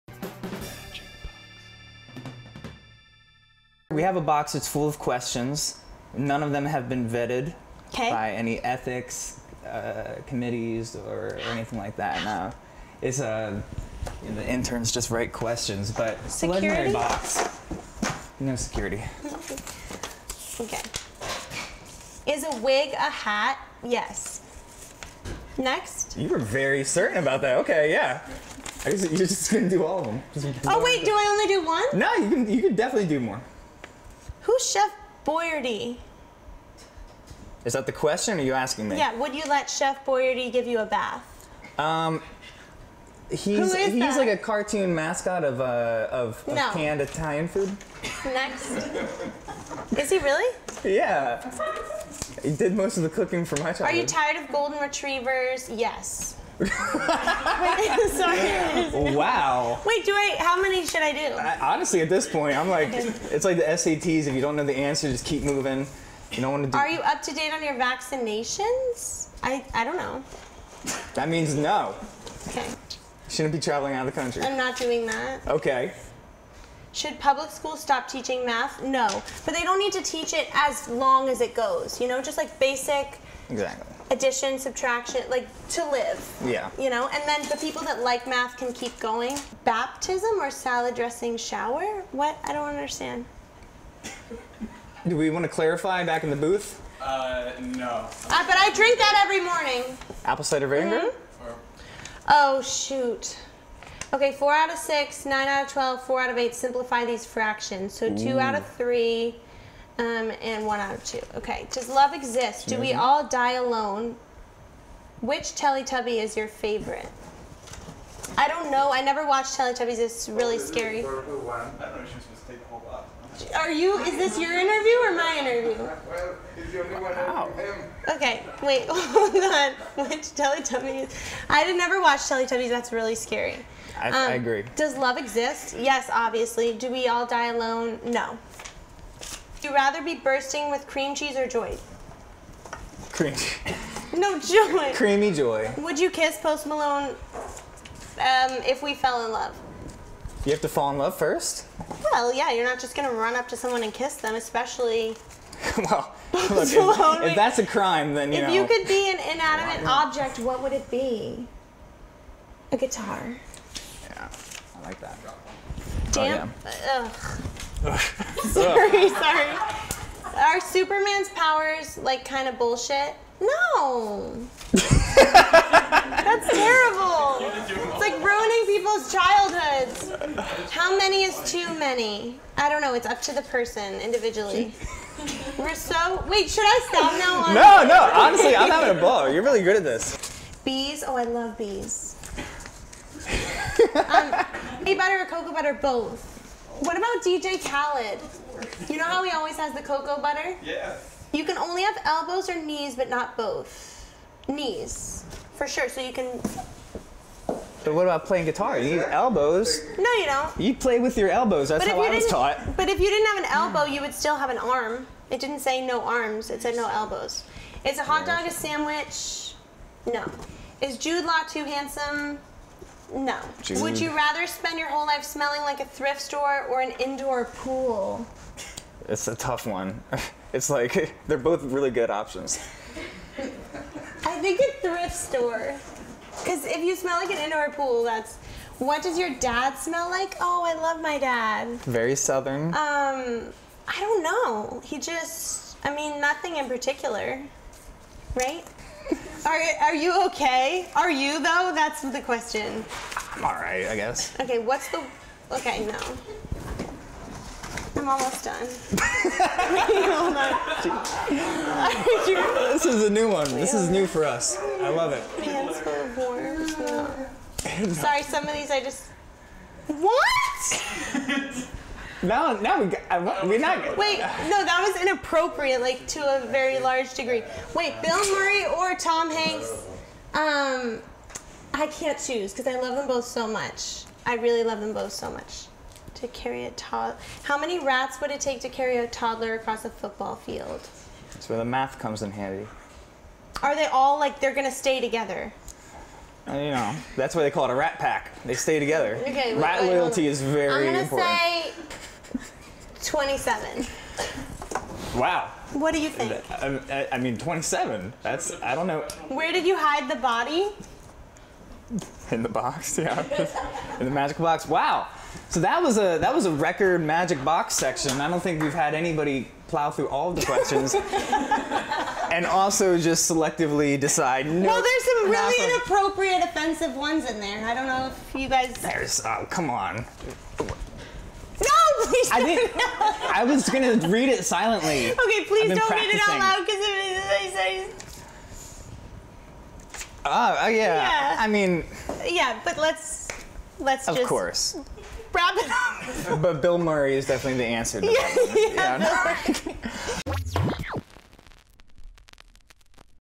The Magic Box Interview with Caroline Jones
Popdust poses wildly embarrassing questions to your favorite artists... and for some reason, they answer them.
Today's brave participant: Caroline Jones